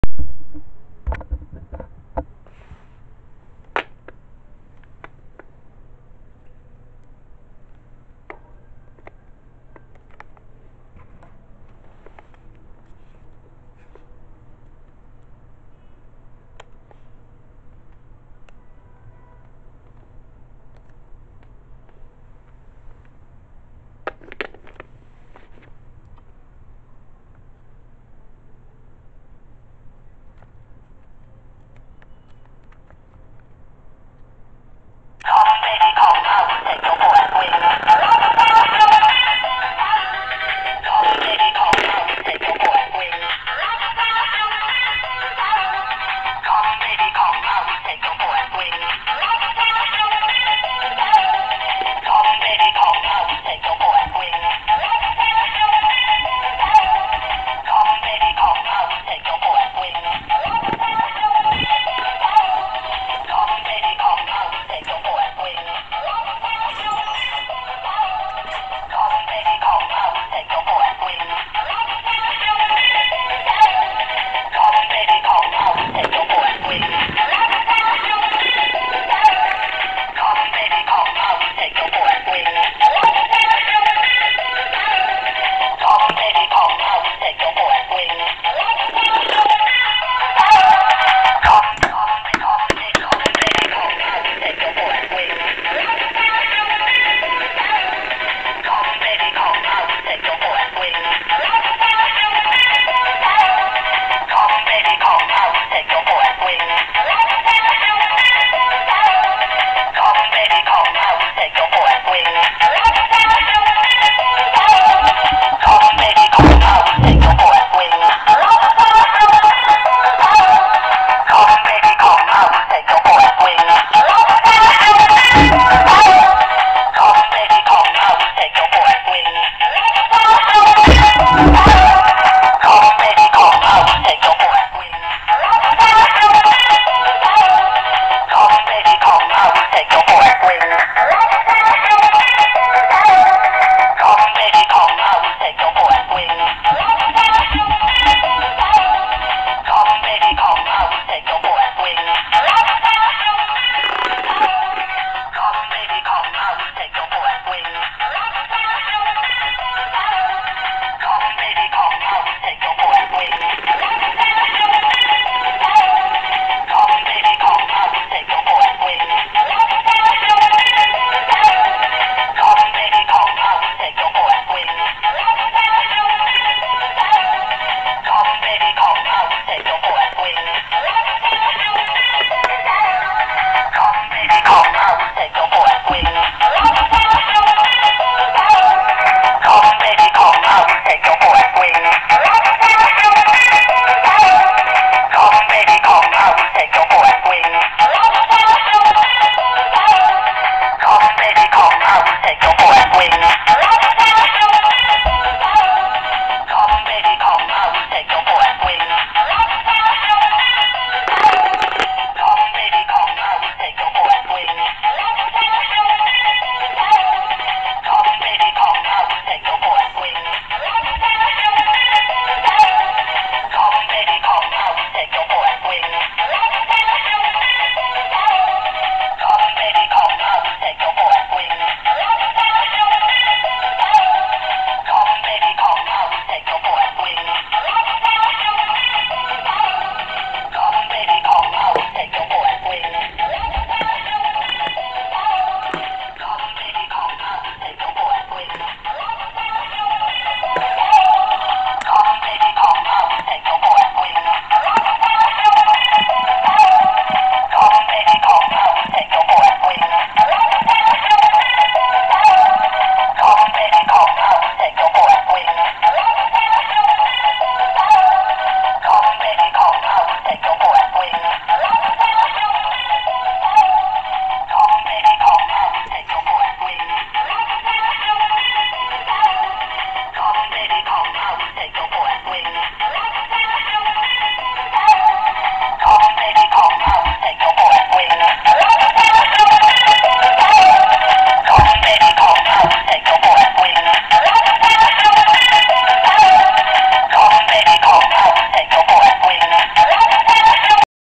Super Top Automatic Musical Flashing Spinning Top Toy
Watch how it looks and spins with a flashing fan!